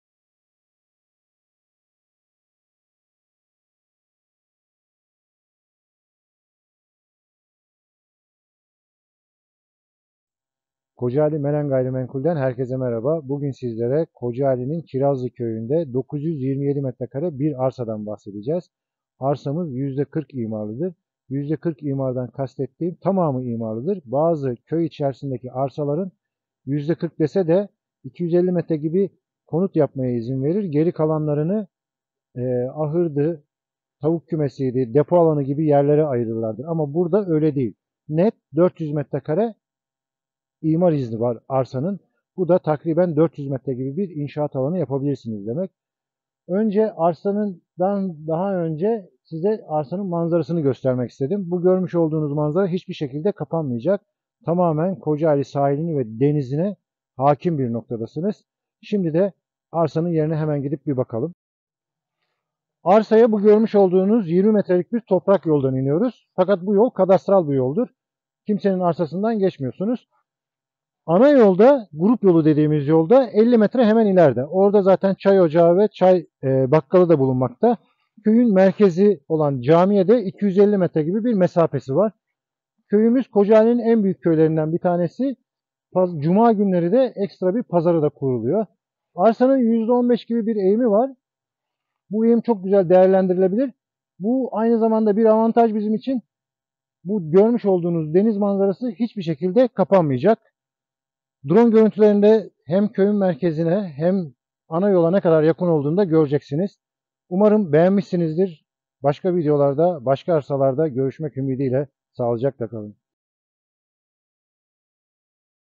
SATILDI KÖYDE DENİZ MANZARALI ARSA MELEN GAYRİMENKUL KOCAALİ 542 264 22 00
Köy içinde, 927 m² kapanmaz deniz ve doğa manzaralı arsa & fındık bahçesi.
Denize 10 dk. mesafede
Konut yapılabilir. 0,40 imarı var.

Kocaali İstanbula 90 dk. Ankaraya en yakın denizi olan Akçakocadan sonra gelen Sakaryanın bir ilçesidir.

KOCAALİ/SAKARYA